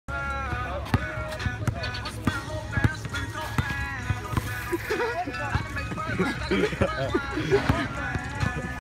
Chadmattbolt silly strings Noah Will
Chadmattbolt escalates his beef with Noah Will by spraying him with silly string